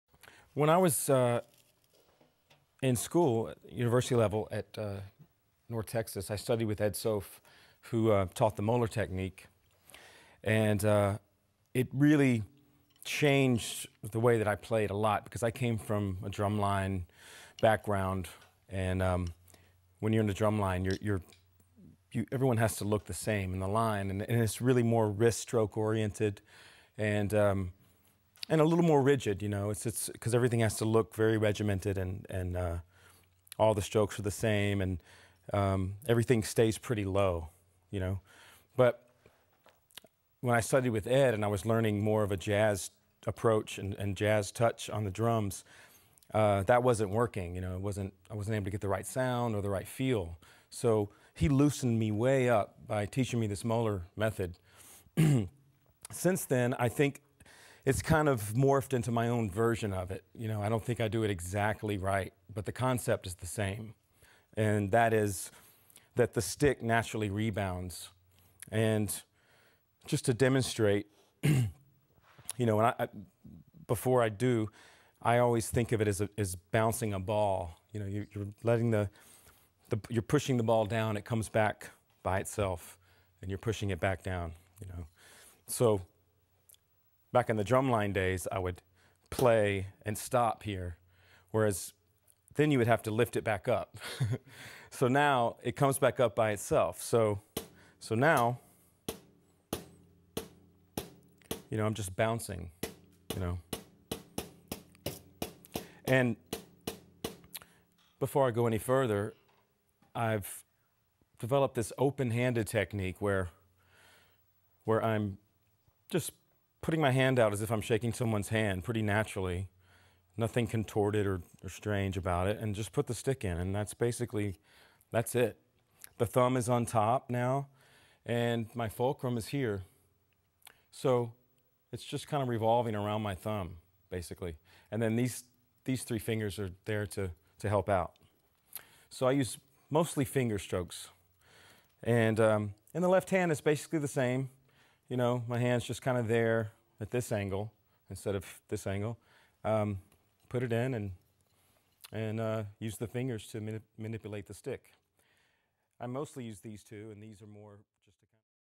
Keith Carlock: Technique Masterclass
Keith talks about developing his hand technique and shows some exercises.
Excerpted from his new DVD - "The Big Picture" - check it out